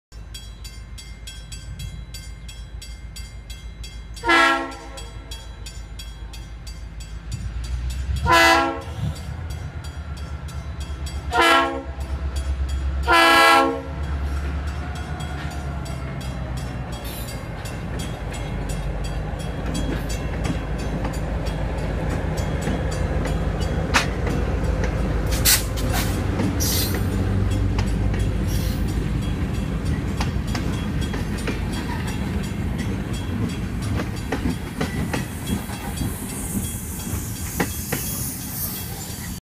7/14/14 - CA-20 mixed freight Norfolk Southern GP38-2's 5290 & 5286
7/14/14 ~7:10 PM  Moorestown, NJ

CA-20 on its way West (North in Conrail terms) back to Camden led by NS GP38-2's 5290 & 5286.

NS 5286 GP38-2 Operation Lifesaver